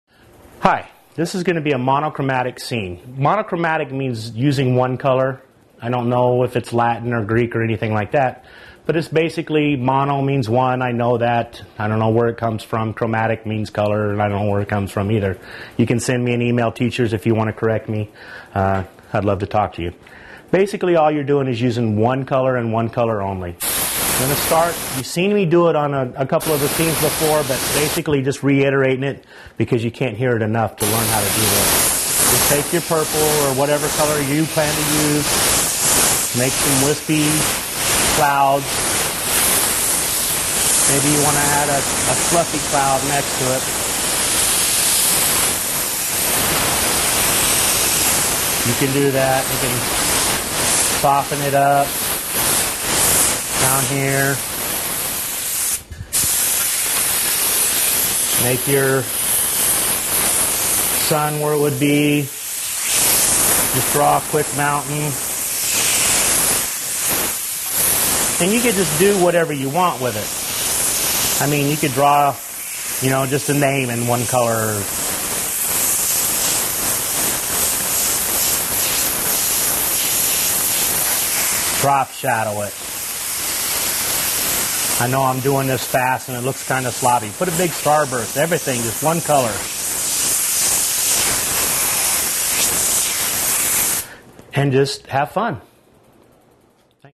Airbrush Desert Scene: Monochromatic
Airbrush Desert Scene: Monochromatic. Part of the series: How to Airbrush a Desert Scene. Painting a desert scene in one color can have a dramatic effect. Learn how to airbrush a desert scene on a T-shirt in this free hobby video.